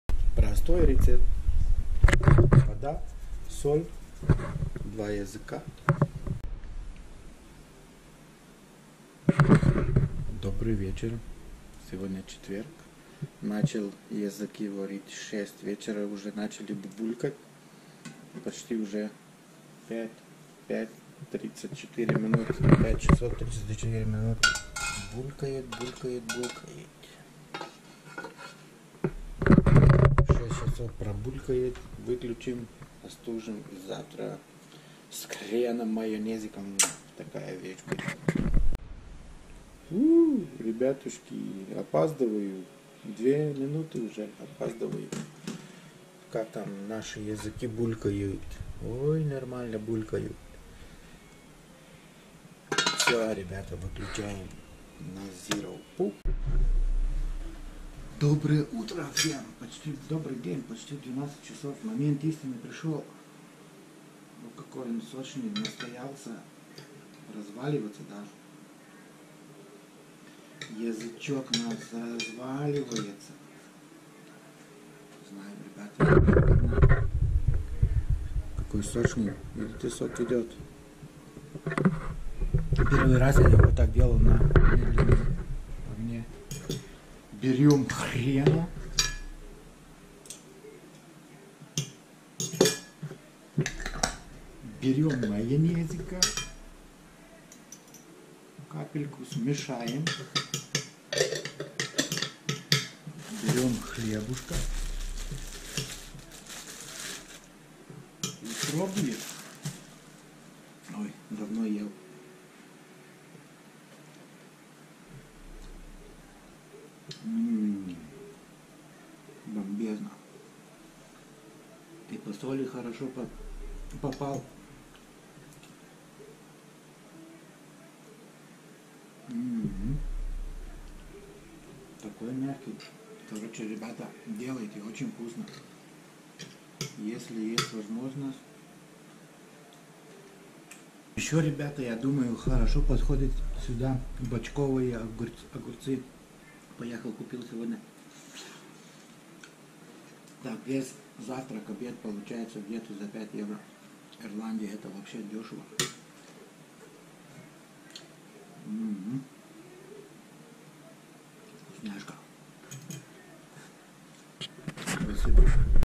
Простой рецепт свиной язык
A simple pork tongue recipe
Water,salt,3 hours very slow boiling temperature.
eating with horseradish and mayonnaise.
Super cool and delicious food.
I cook 6  hours is too much